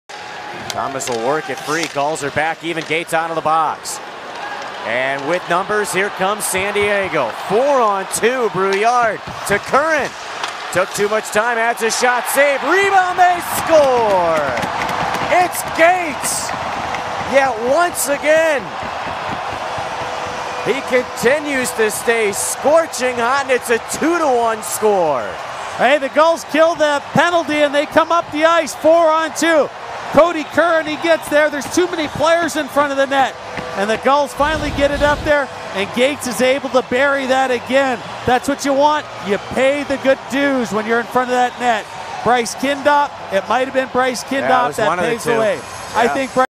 Kindopp Crashes The Blue Paint
Bryce Kindopp goes to the dirty areas, parking in front of the crease to score his